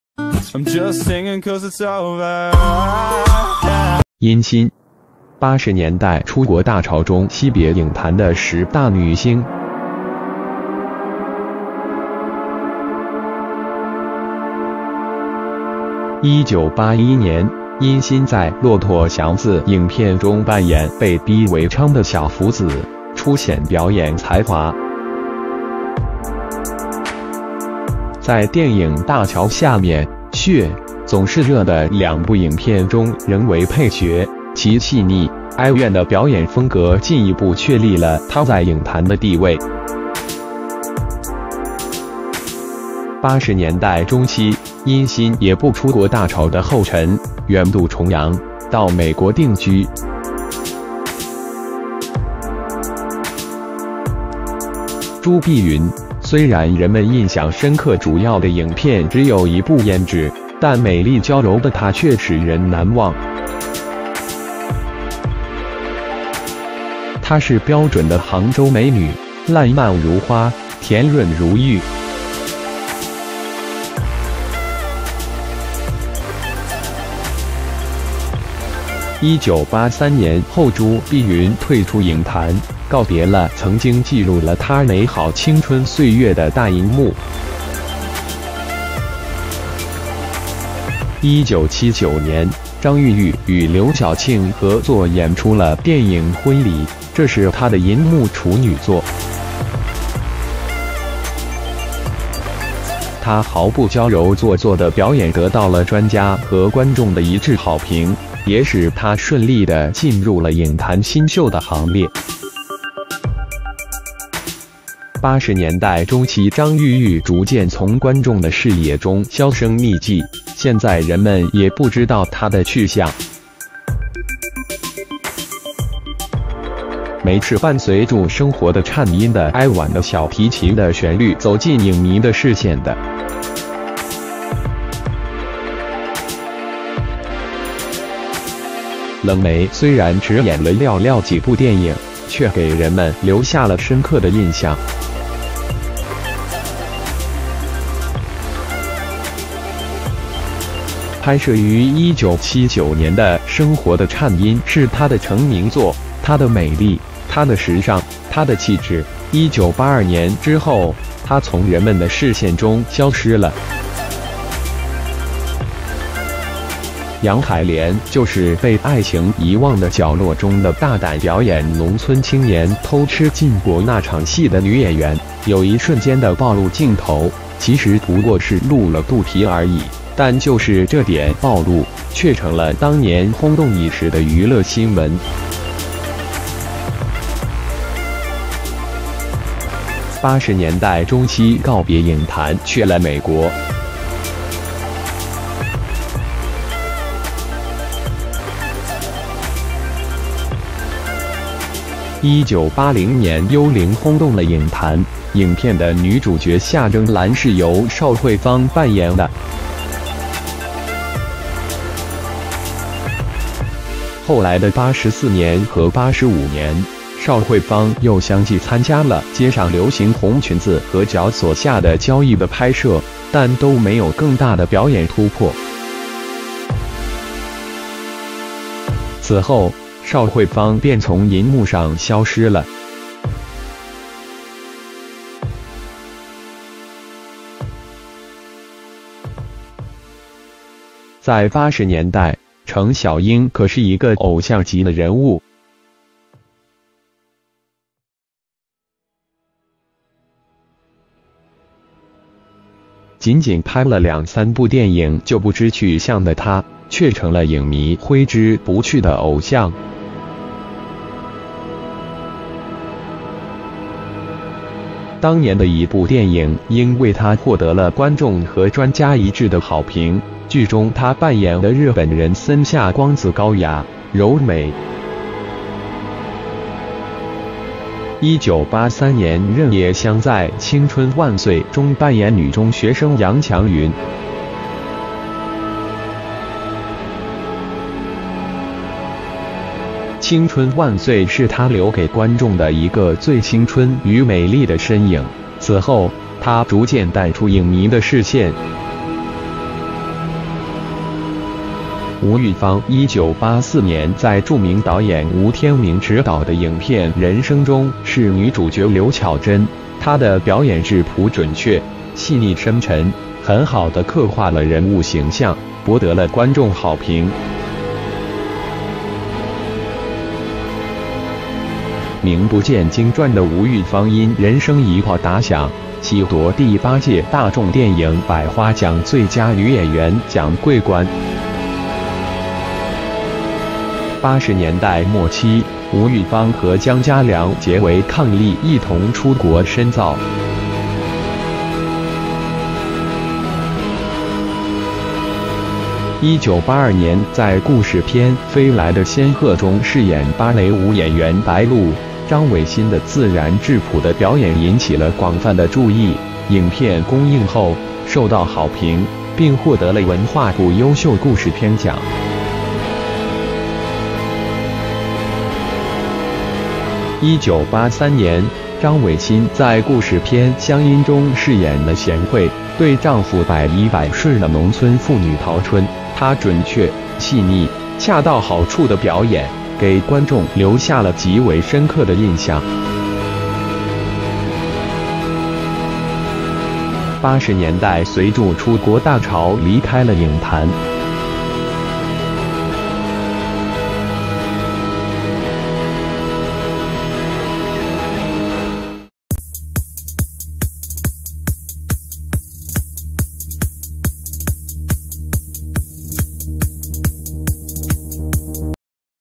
[殷新] 80年代出國大潮中惜別影壇的十大女星 | HK Gossip 24.7
[殷新] 80年代出國大潮中惜別影壇的十大女星